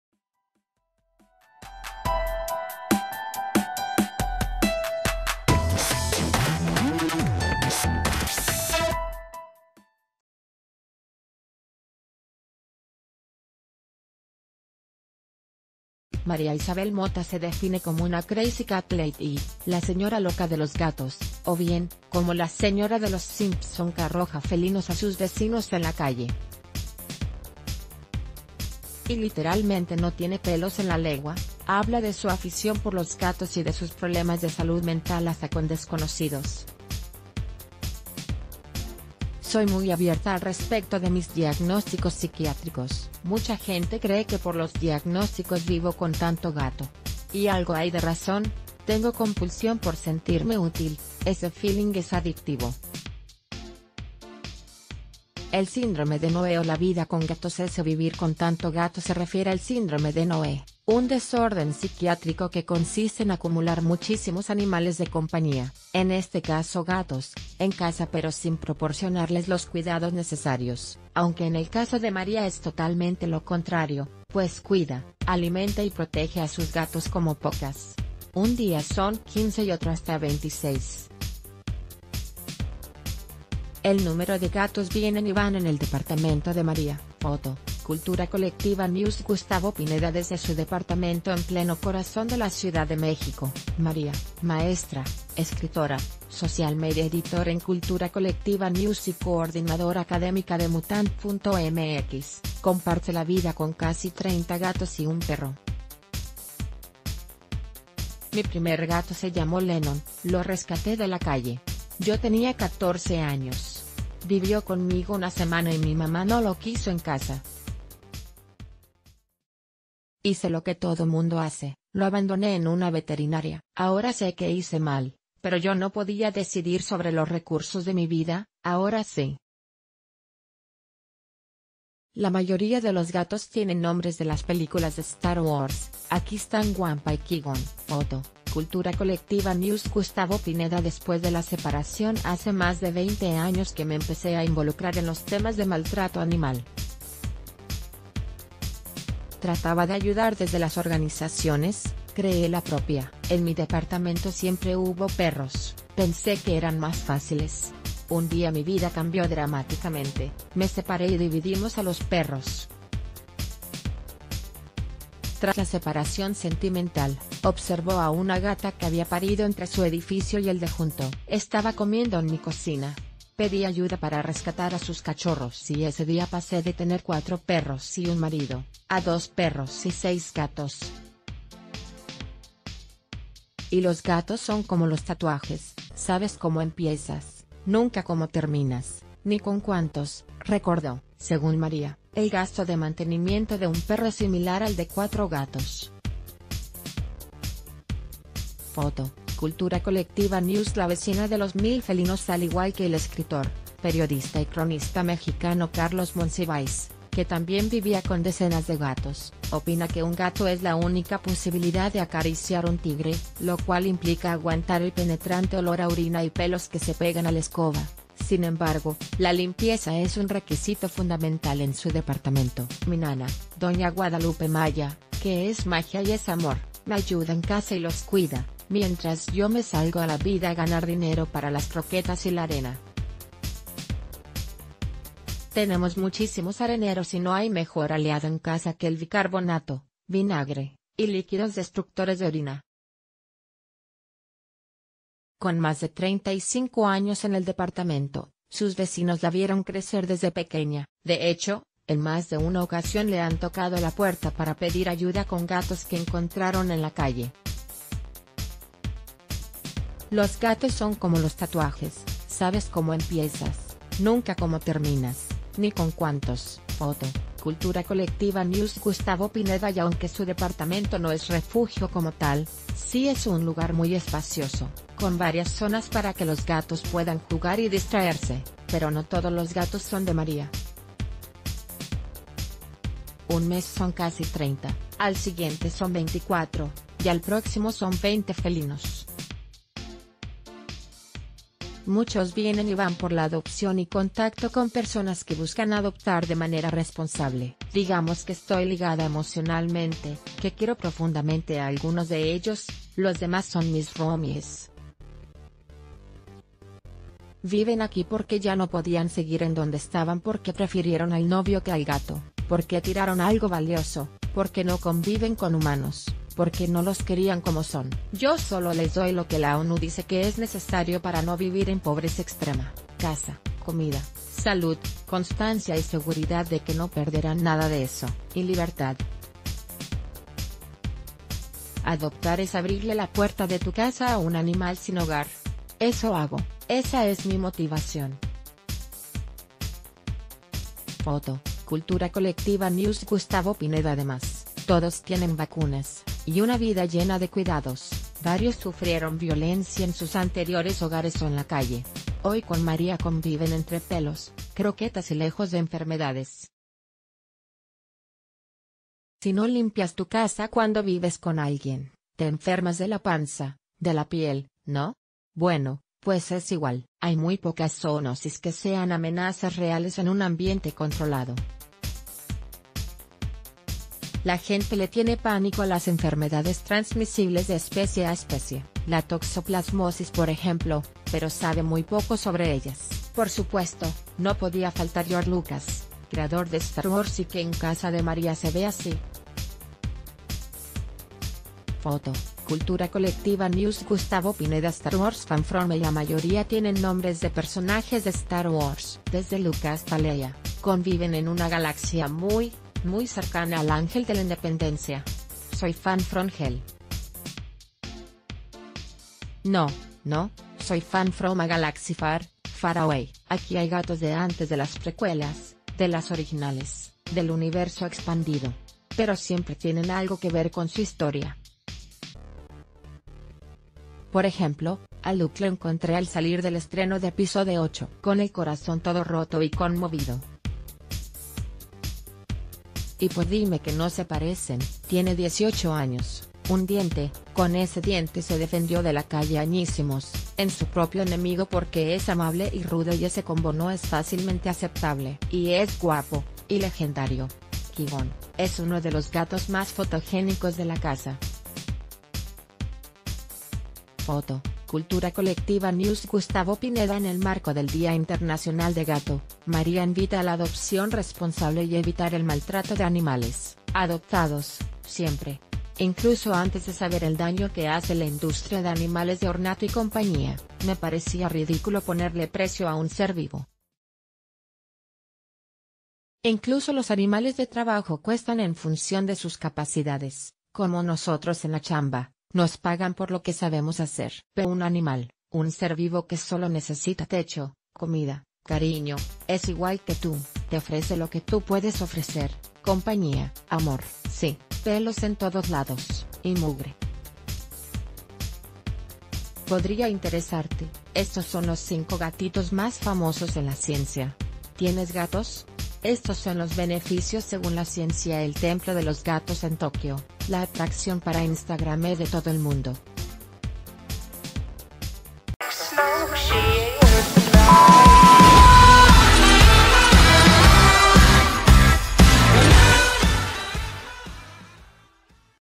‘Amo tanto a los gatos, que vivo con casi 30’
Mariaisabel Mota se define como una crazy cat lady (la "señora loca de los gatos") o bien, como la señora de los Simpson que arroja felinos a sus vecinos en la calle. Y literalmente no tiene pelos en la legua, habla de su afición por los gatos y de sus problemas de salud mental hasta con desconocido...